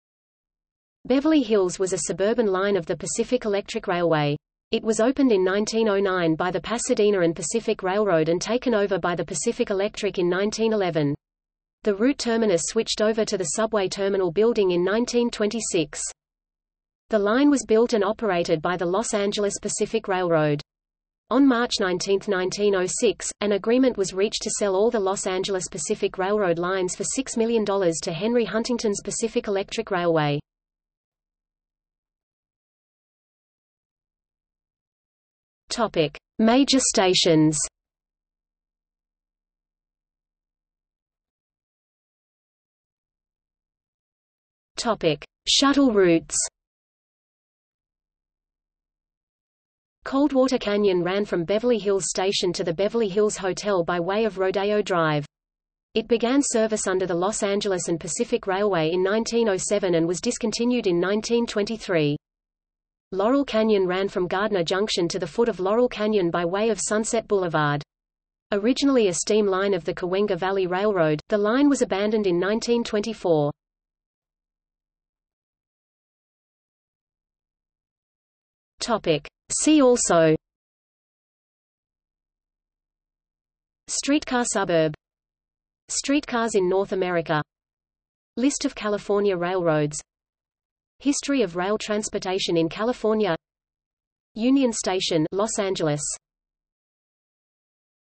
Laurel Canyon (Pacific Electric) | Wikipedia audio article
00:00:32 1 Major Stations
00:00:41 2 Shuttle Routes
00:01:15 3 See also

- Socrates

SUMMARY
Beverly Hills was a suburban line of the Pacific Electric Railway. It was opened in 1909 by the Pasadena and Pacific Railroad and taken over by the Pacific Electric in 1911. The route terminus switched over to the Subway Terminal Building in 1926.
The line was built and operated by the Los Angeles Pacific Railroad. On March 19, 1906, an agreement was reached to sell all the Los Angeles Pacific Railroad lines for $6 million to Henry Huntington’s Pacific Electric Railway.